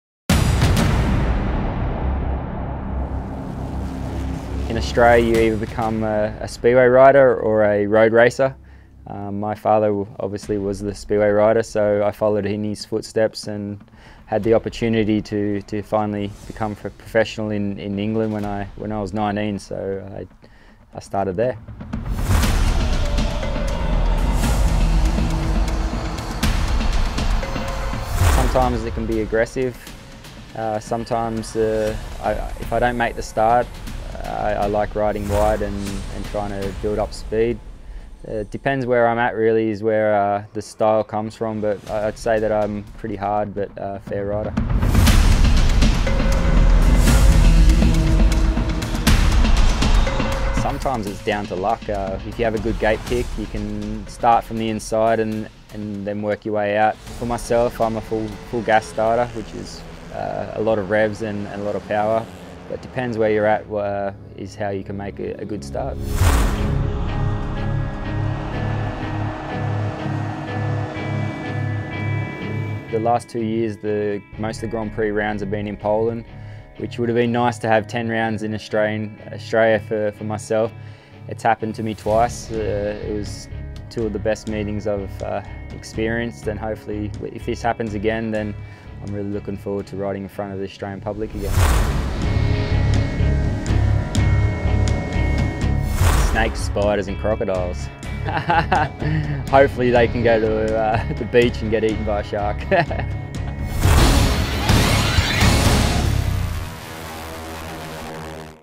The Profile of Jason Doyle | FIM Speedway Grand Prix
Following in his father's footsteps, Jason Doyle talks about his route to Speedway and how he goes about a race weekend in the championship 🏍